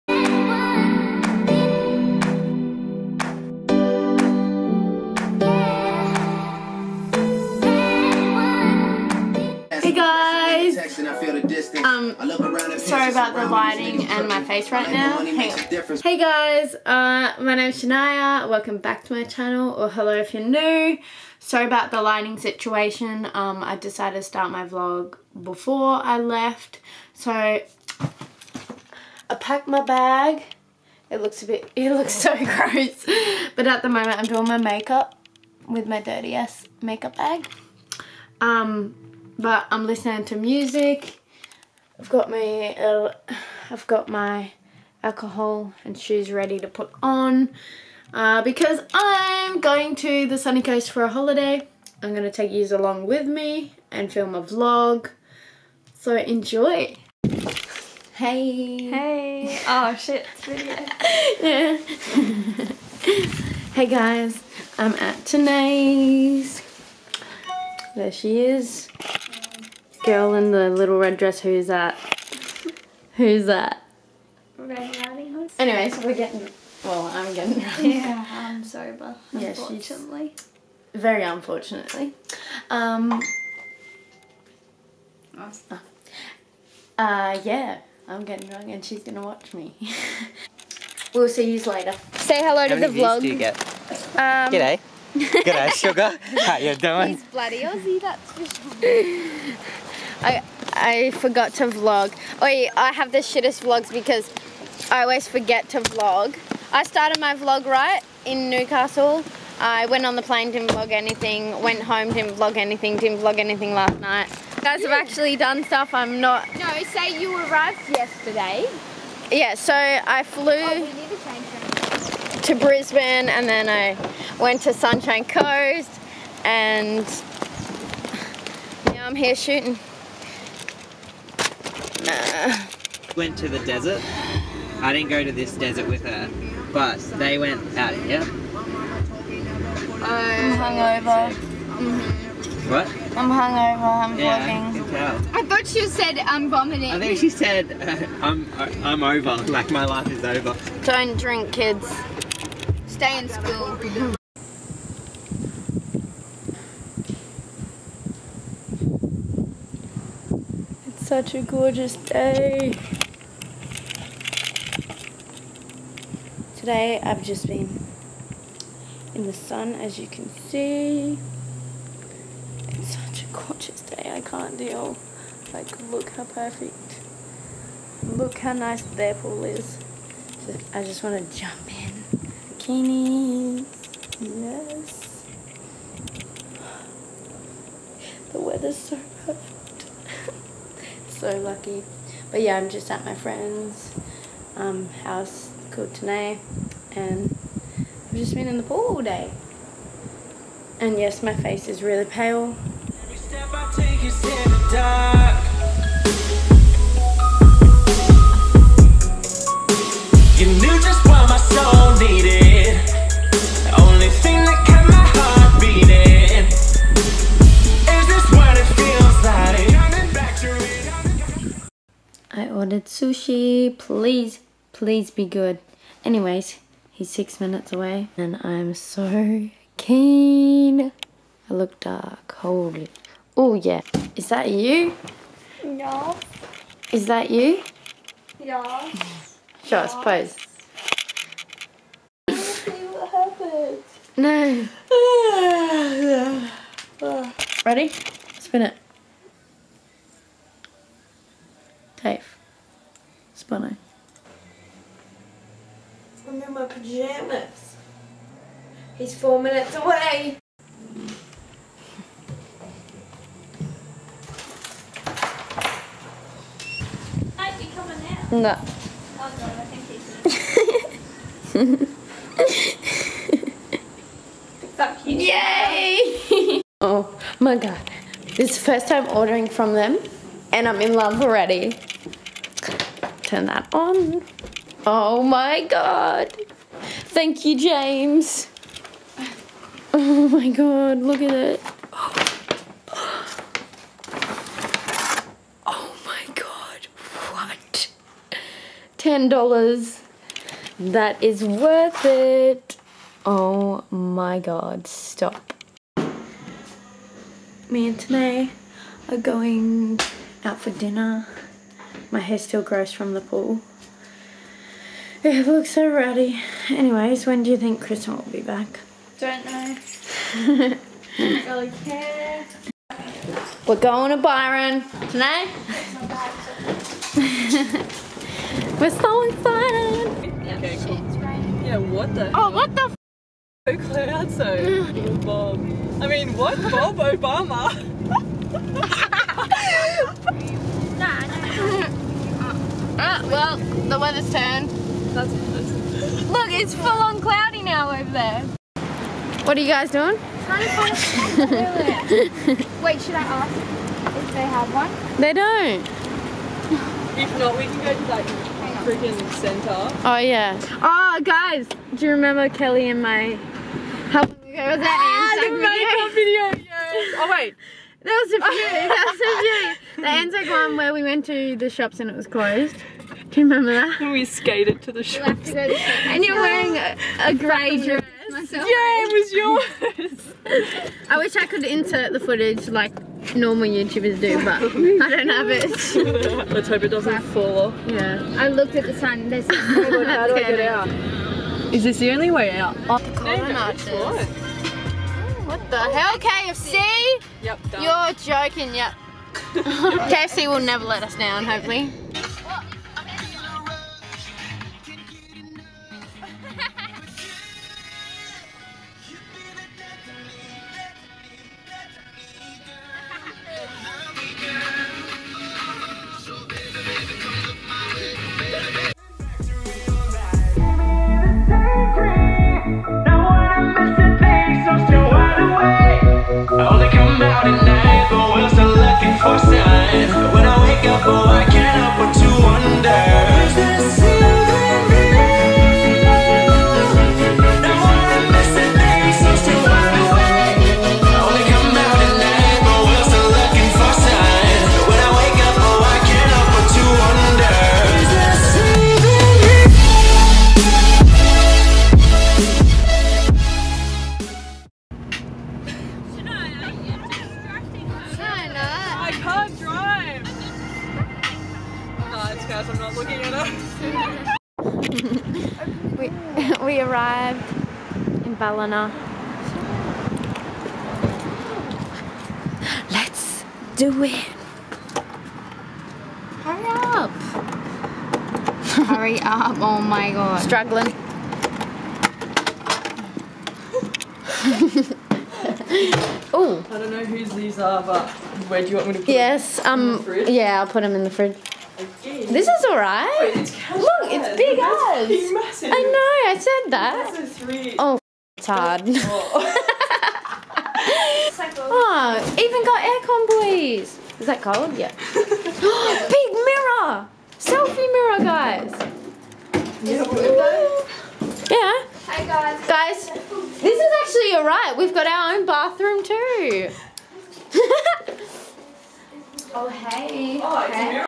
Byron Bay Girls Trip | Part One
Hey guys! I recently went on holiday and what better way to celebrate then go on a Byron Bay girls trip!!!
We had so much fun and I hope you enjoy this vlog
Let me know if you like these kinds of videos!

Love Shaniah xxx